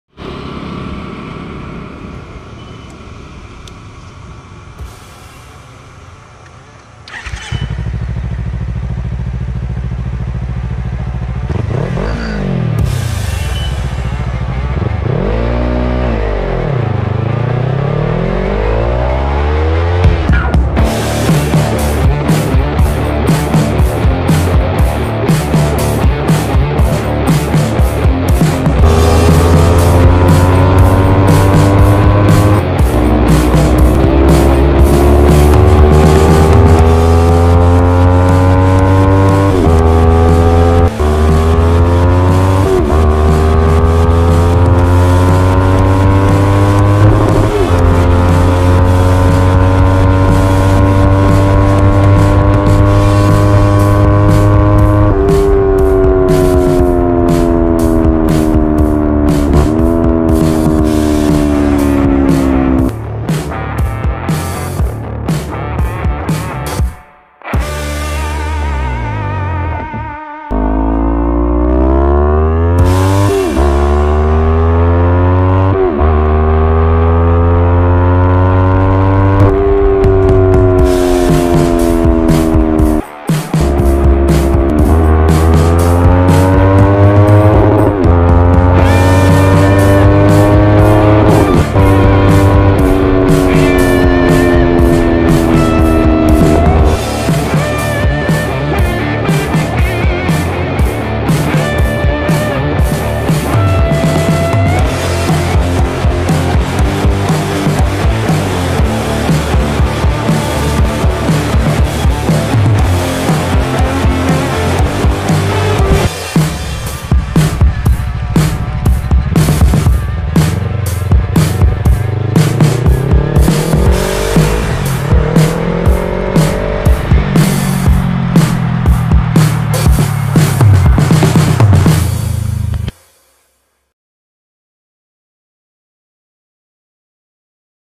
Best Mounting Position GOPRO camera on Motorcycle
My favourite sport to mounty GoPro hero 7 black on my ninja 300,
coz it capture all my riding position as well as background view .